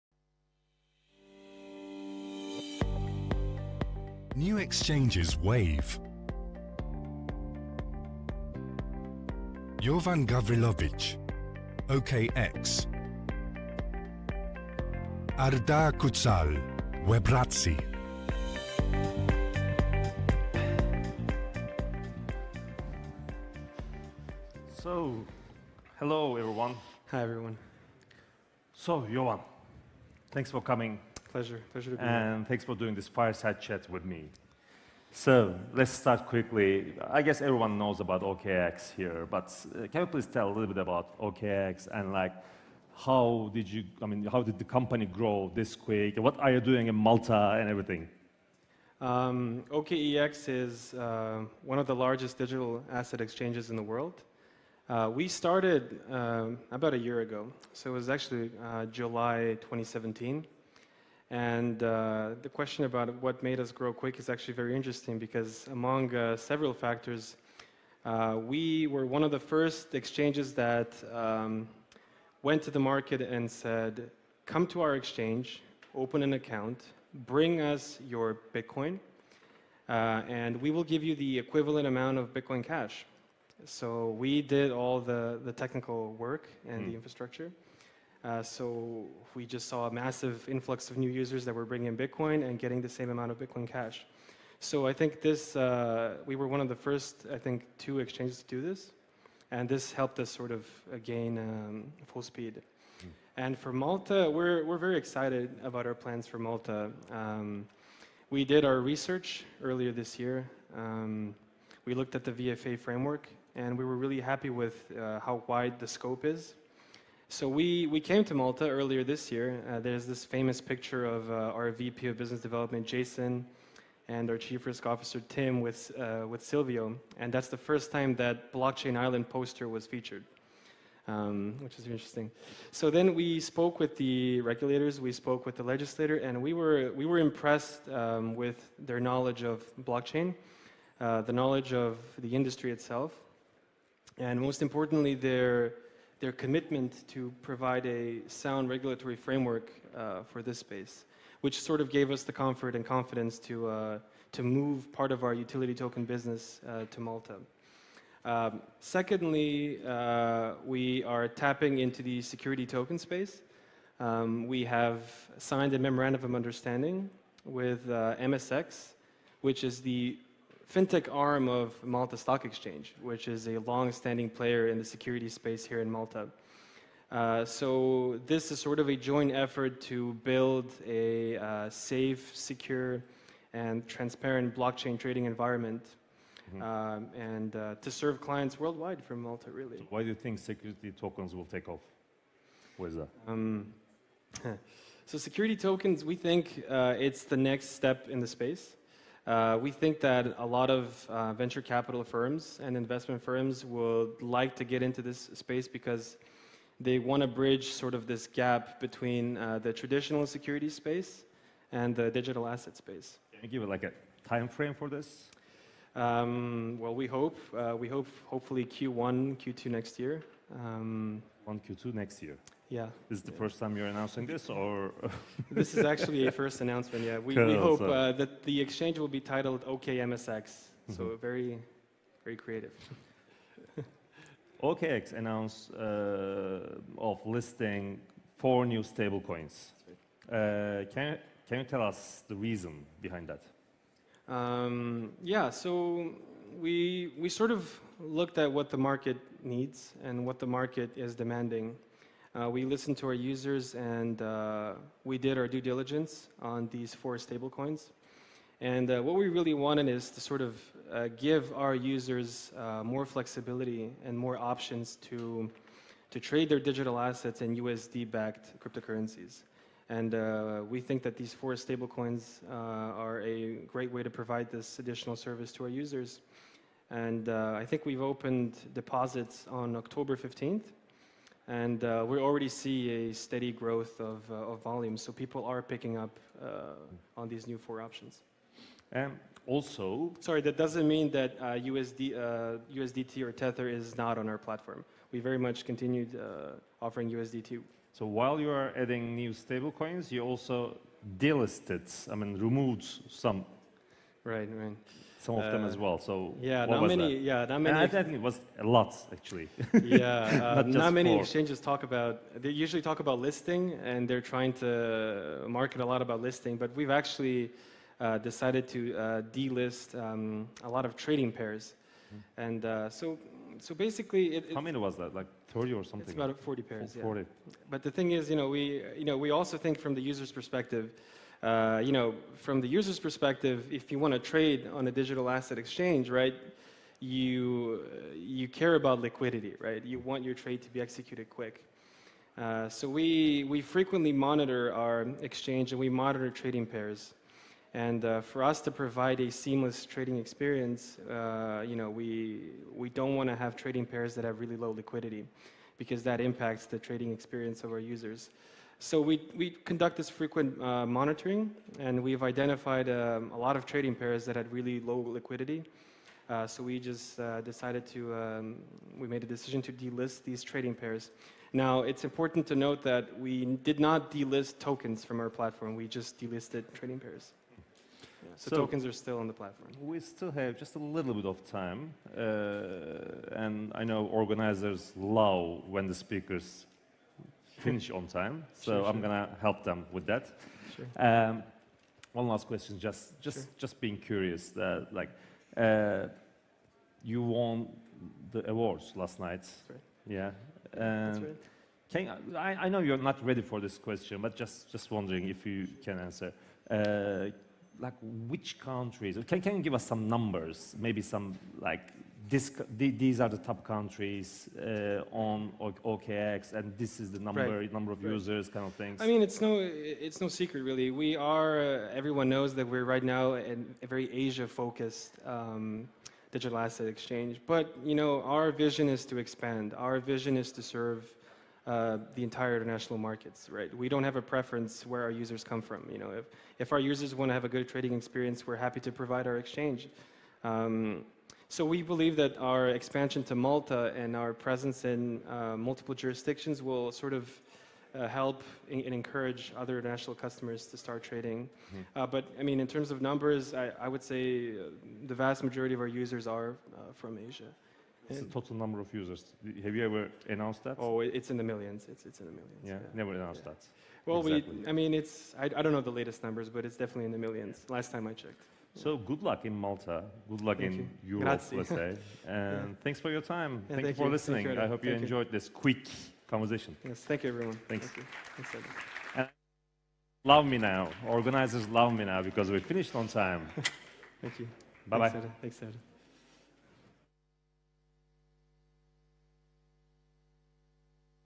A fireside chat during
Learn more about @OKEx plans for Malta with @jovangvc in a fireside chat during @BlockchainMT

Source: Malta Blockchain Summit